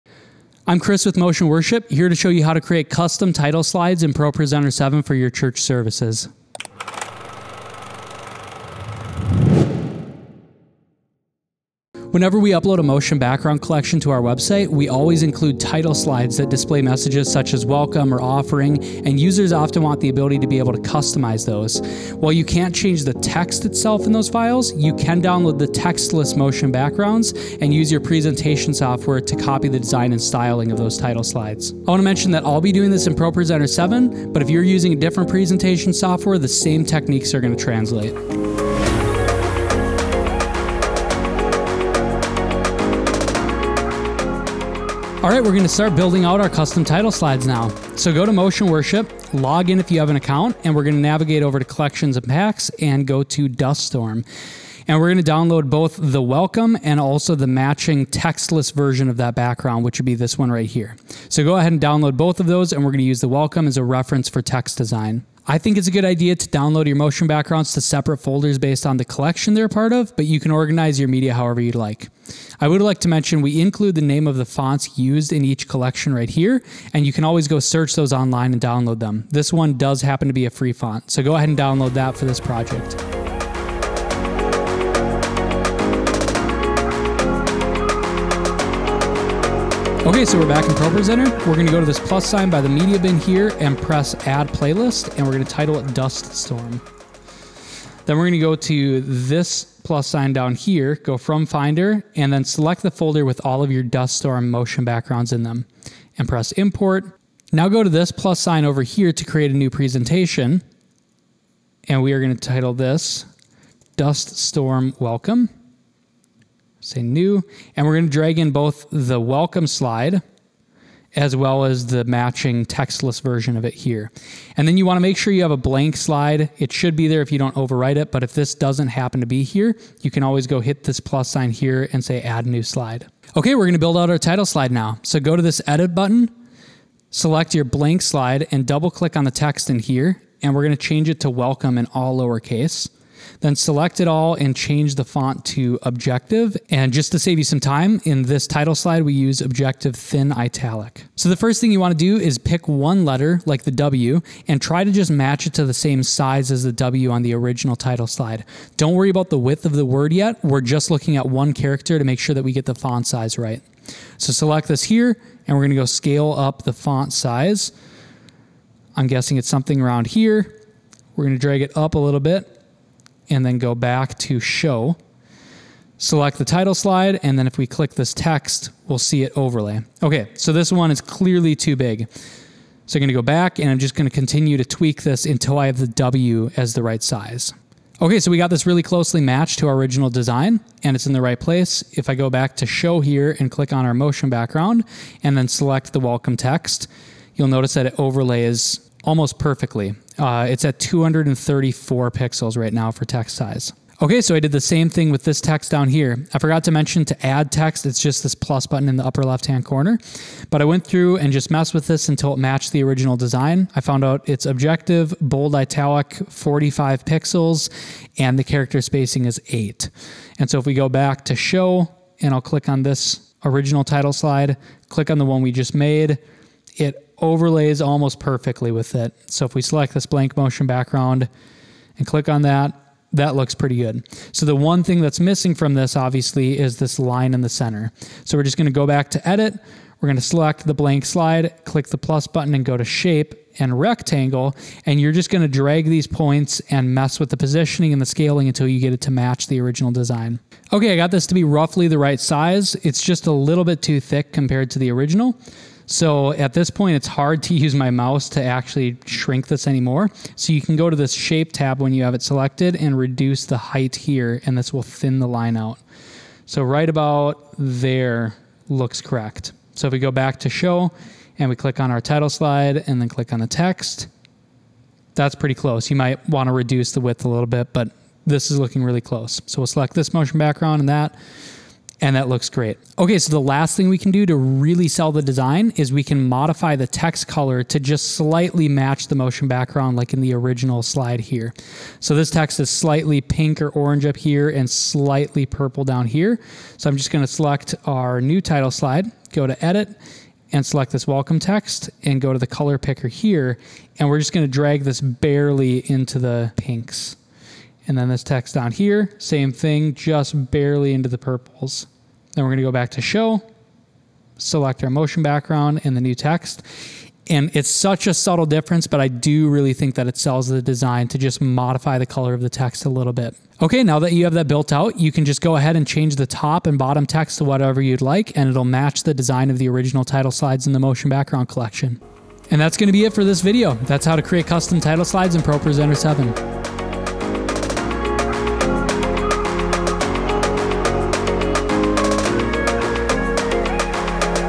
Custom Title Slides | ProPresenter in 5 Minutes
This “ProPresenter in 5-minutes” series is hosted by Chris Fleming, Motion Worship Creative Director, professional drummer and music director from Minneapolis, MN. In this collection of concise tutorials you will learn about the ProPresenter interface, how to customize slides, build a library of songs, compile a full church service, and tons more!

In this specific video tutorial, Chris will walk you through how to customize title slides with your own text in order to match an existing collection of graphics.